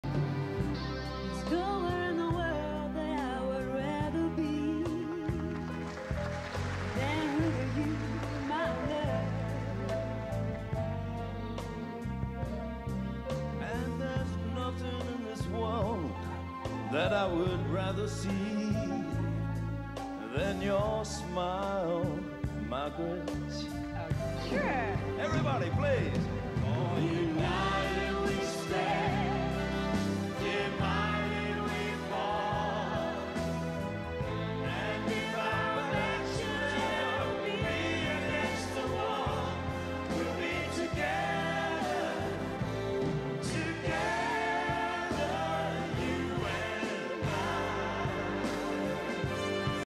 Rest in Love Margaret Urlich
RIL Margaret Urlich. Margaret with Tama & Temuera Morrison 1994 Entertainer Awards